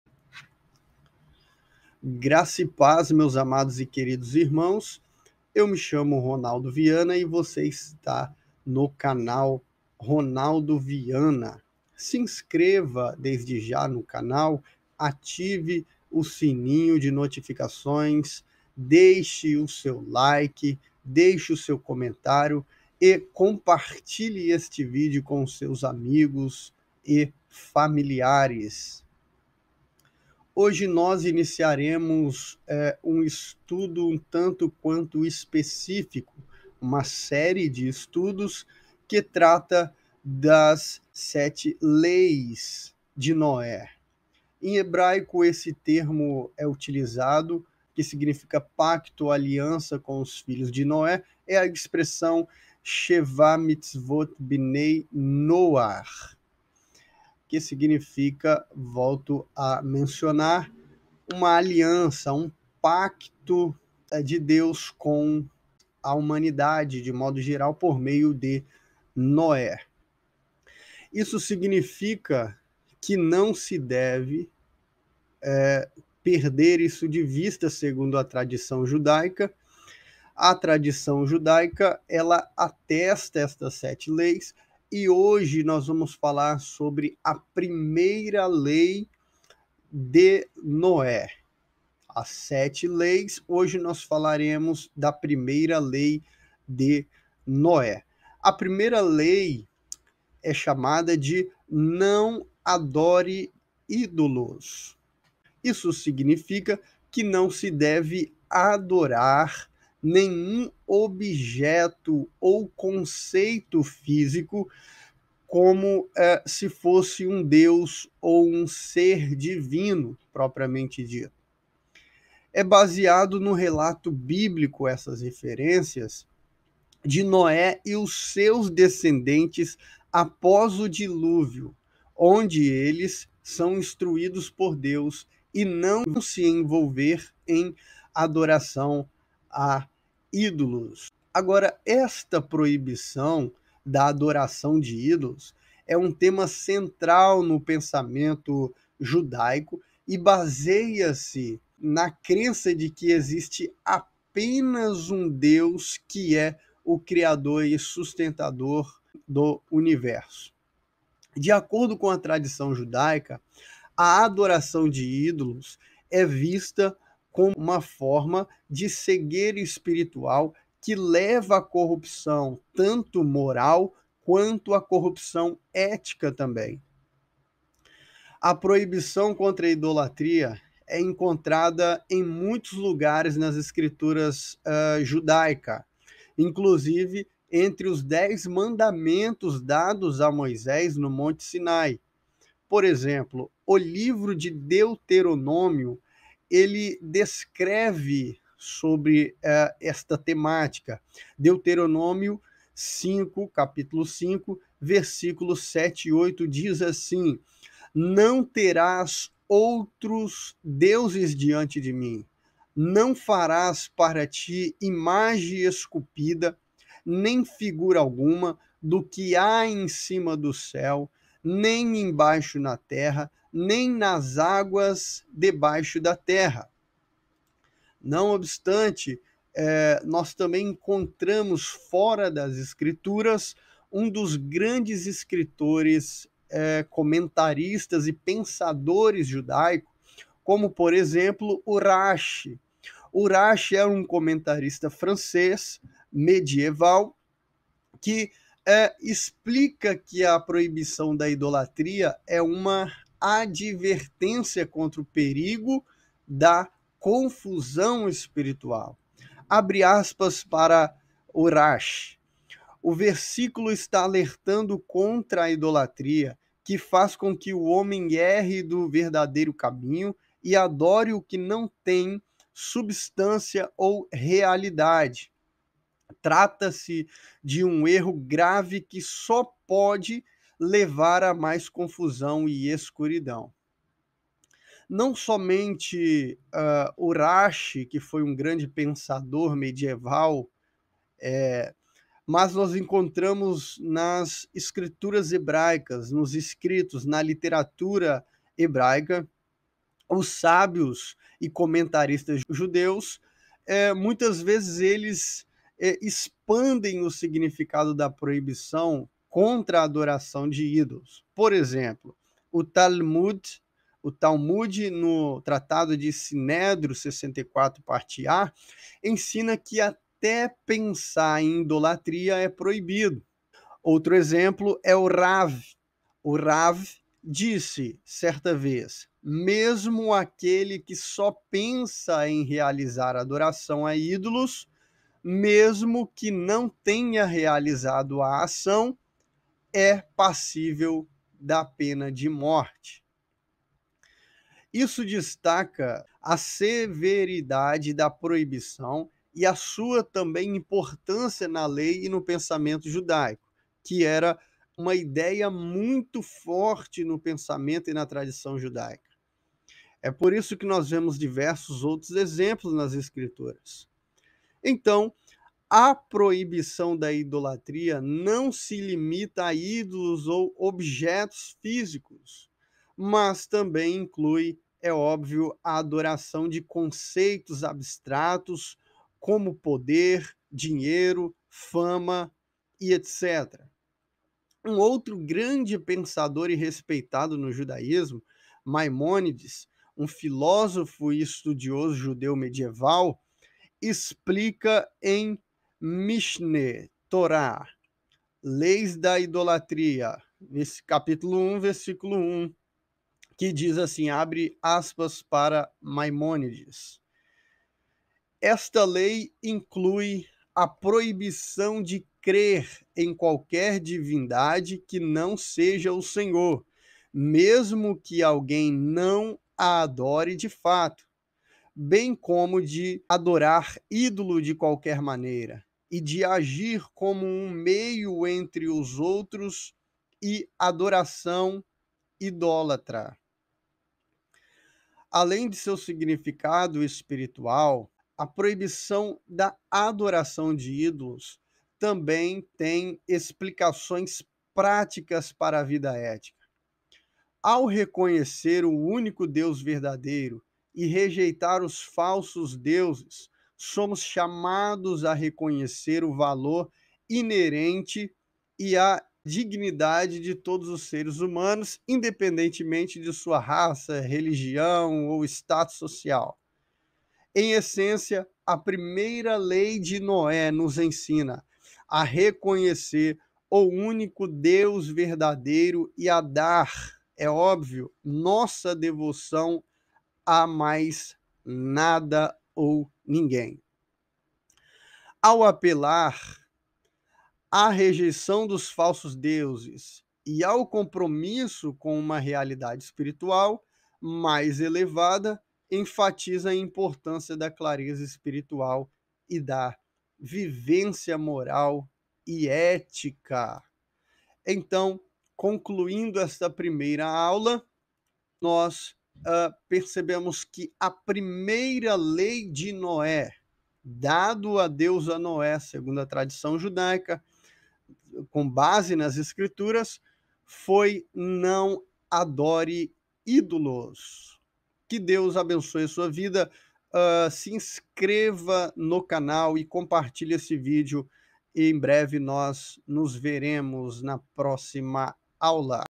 As Sete Leis de Noé | Primeira Lei | Ronaldo Viana
Olá meus amados irmãos,graça e paz. Estimo que estejam bem!
Hoje daremos início a uma série de estudo sobre as sete leis de Noé. Cada aula uma Lei.

Curtem..
Comentem..
Compartilhe e ative o sino de notificações.